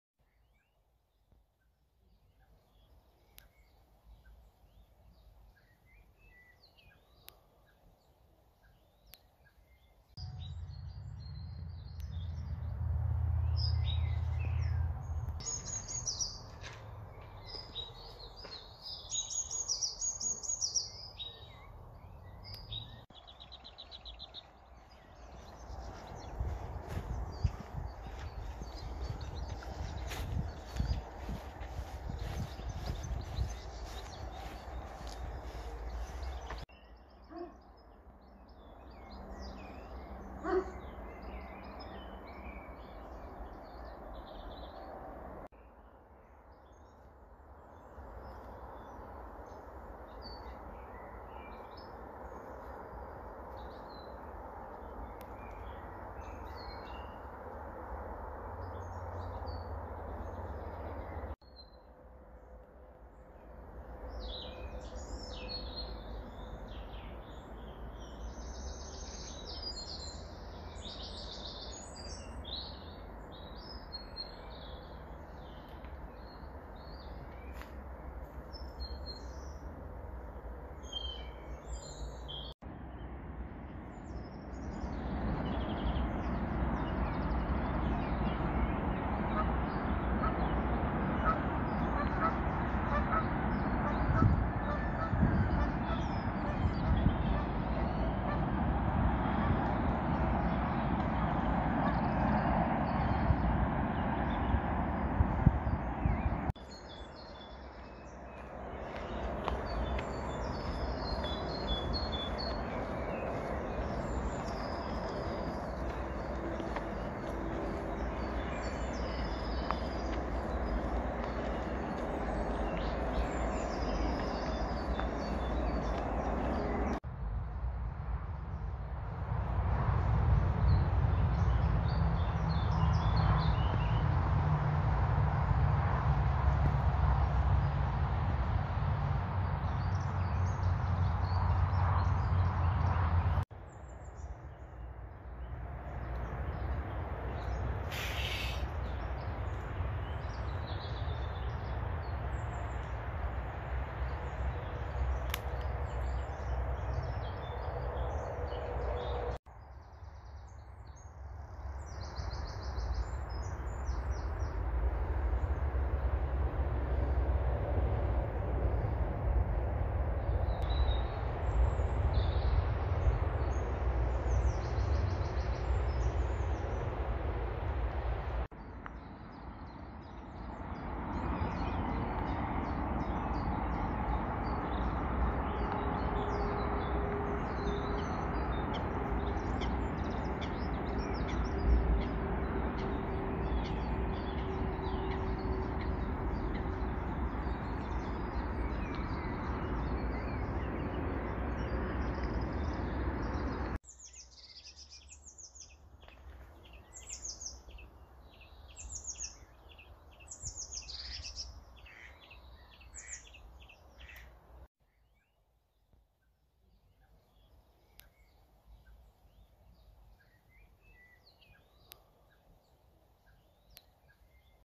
Turnberry Estate to Sneyd Reservoir, 26th April 2020
A Sunday stroll in Bloxwich.